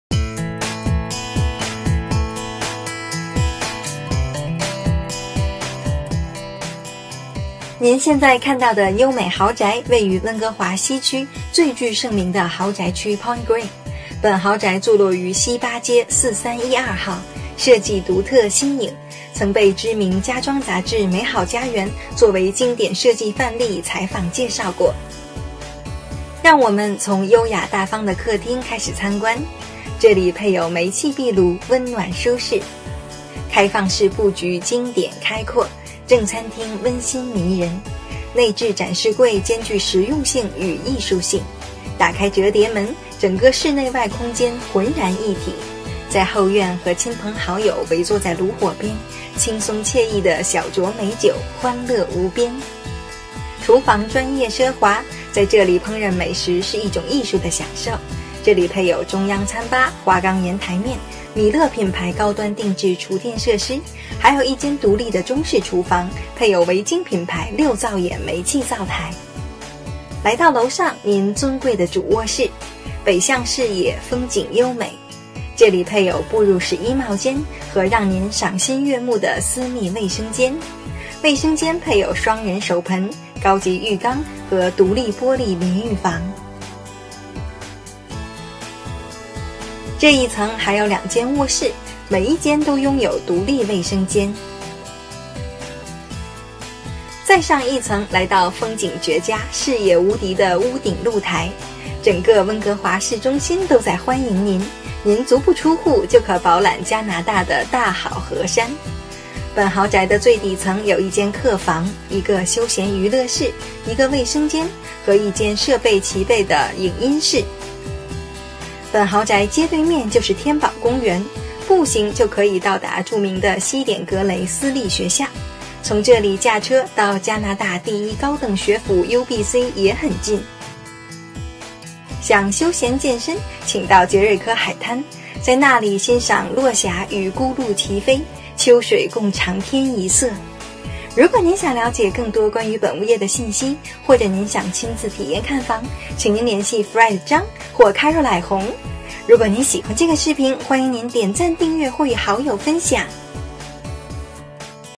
4312 W 8th Avenue, Vancouver - Mandarin
Gorgeous OCEAN MOUNTAIN and CITY VIEW. Contemporary home at sought after West Point Grey. Architecturally stunning and modern living experience. Designed by renowned Georgie Award/IDIBC award winner John Henshaw Architect Inc. This ONE OF A KIND CUSTOM BUILT home offers open concept living space and kitchen w/ 10' ceilings & private decks/lush terraces for indoor/outdoor entertaining in private southern backyard. With 3 spacious bedrooms upstairs and a roof deck patio that capture the panoramic view. Top quality, great craftsmanship, streamlined gourmet kitchen w/ lrg island, Viking, Miele appl, beautiful custom cabinetry & millwork, and much more! Right across from Trimble Park, WPGA, and minutes away from UBC, and Jericho Beach